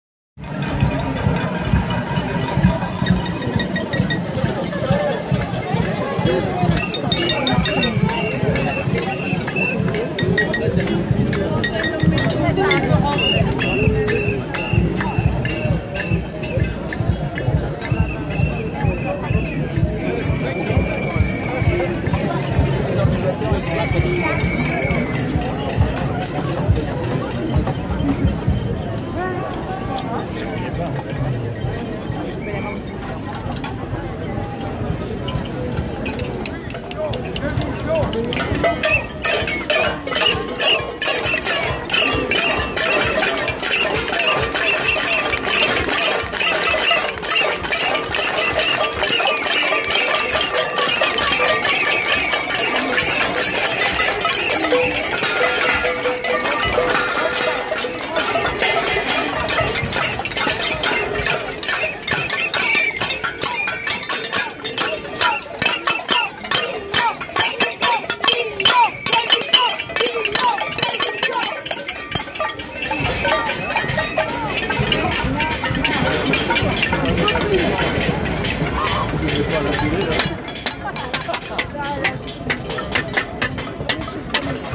Son des casseroles contre la coruption Fillon démission
Paris, France le 5 mars 2017 - Rassemblement, Place de la Republique, pour le respect du peuple, de la justice et de la presse, pour contrer le rassemblement du Trocadero.
Demonstration, Place de la Republique, for the respect of the people, the justice and the press, to counter the demonstration of the Trocadero.